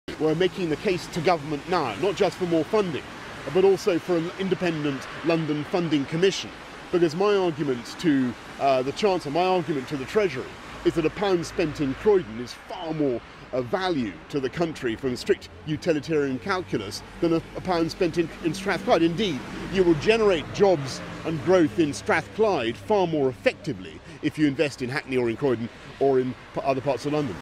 Boris Johnson: A pound spent in London is of more value than a pound spent in Strathclyde
Brexit campaign leader Boris Johnson demonstrates his support for Government investment in London rather than in Scotland. Leave campaign promises that Westminster will increase Scotland's budget after a Brexit are undermined by the stated investment policy of the most likely next PM.  It is also the case the Norway who access the common market through an EFTA trade deal pay 95% of the UK's per head membership fee in trade levies but get no EU funding so there is no membership fee saving anyway unless you don't do a deal to access the common market and then there would be tariffs and higher prices on imports and exports.